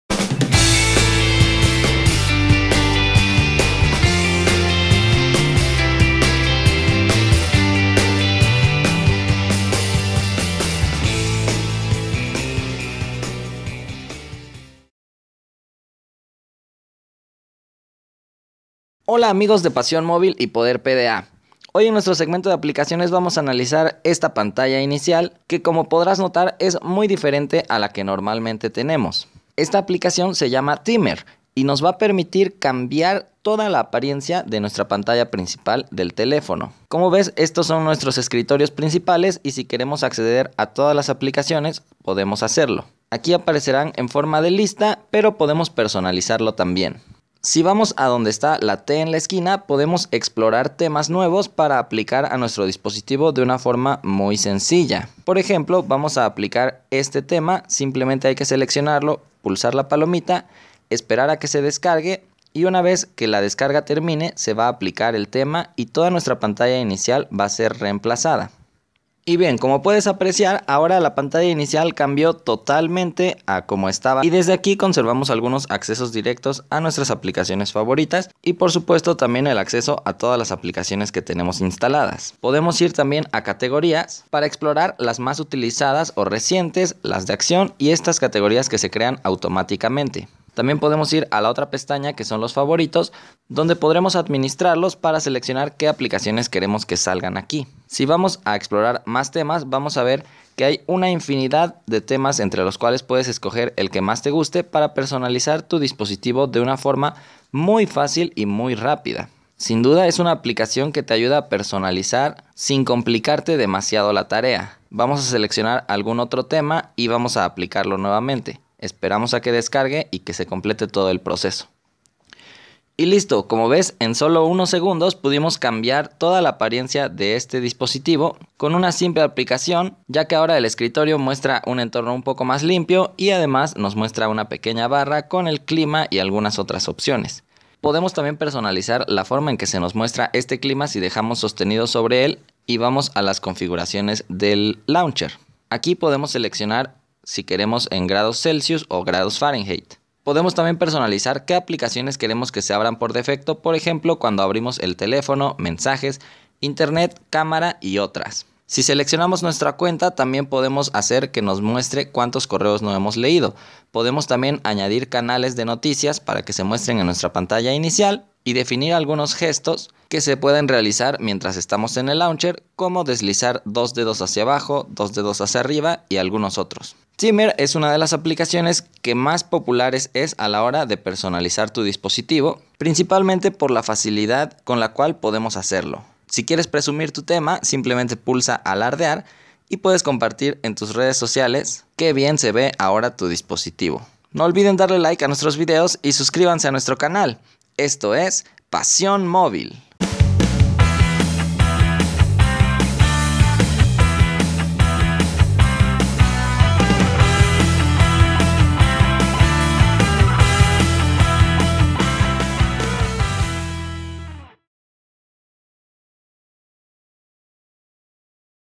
Themer en Español
Ahora te explicamos la aplicación "Themer", que básicamente trata sobre una forma de personalizar a fondo nuestro smartphone con un interesante skin.